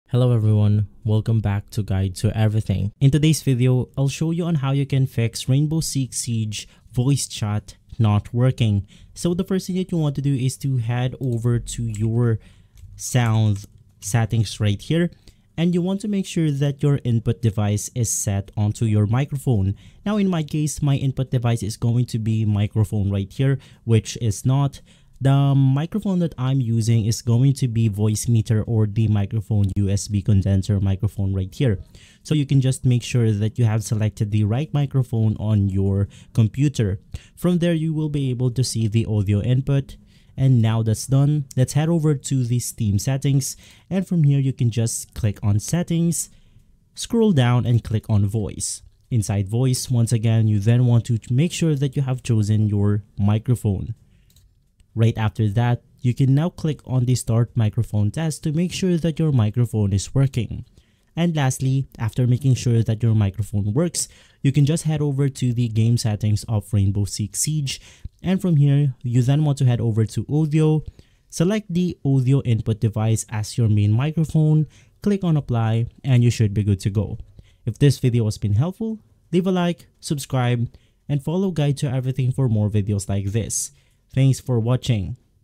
How to Fix Voice Chat Not Working in Rainbow Six Siege (2026 Guide)
Hello in this video I will show: How to Fix Voice Chat Not Working in Rainbow Six Siege (2026 Guide)